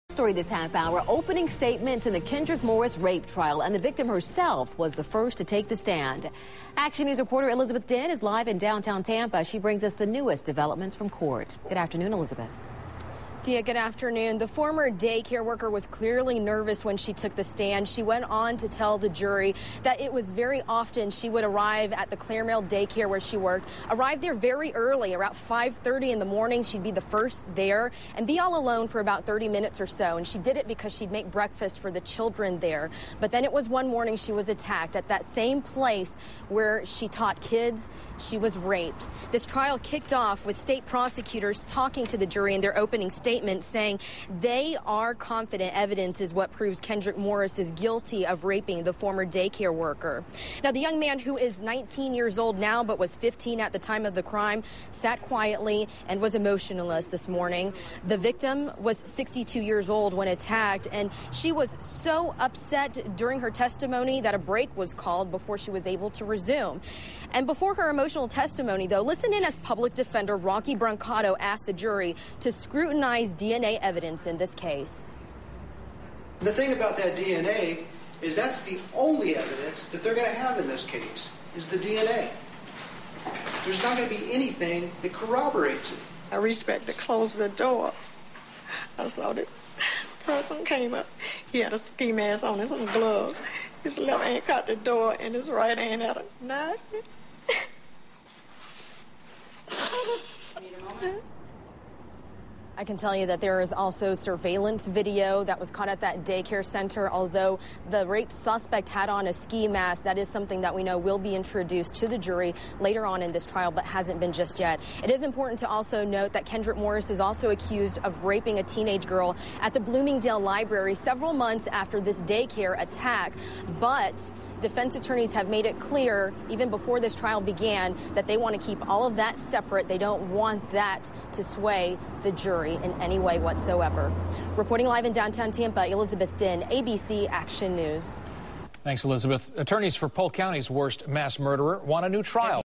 Day care rape victim describes attack
19-year-old Kendrick Morris, who was 15 when the crime was committed, is on trial for raping a 62-year-old day care worker three years ago.